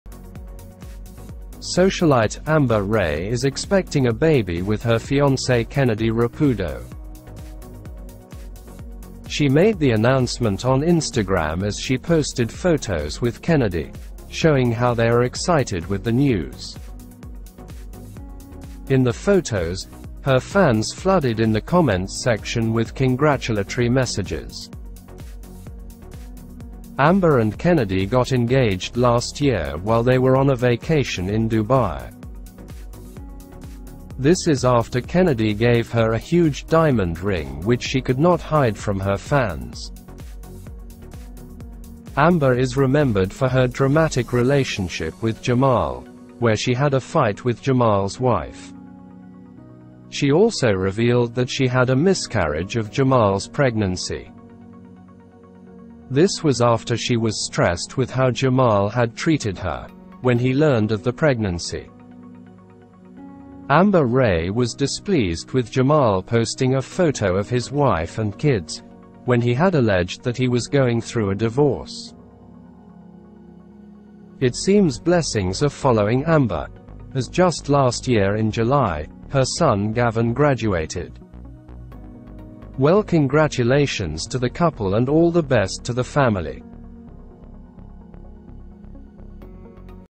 Amber Ray is Pregnant - Expecting Baby with Fiancé Kennedy Rapudo
Socialite Amber Ray has announced she is pregnant and expecting a baby with her fiancé Kennedy Rapudo on Instagram. Amber Ray is remembered for her dramatic relationship with Jamal Roho Safi where she fought with his wife Amir.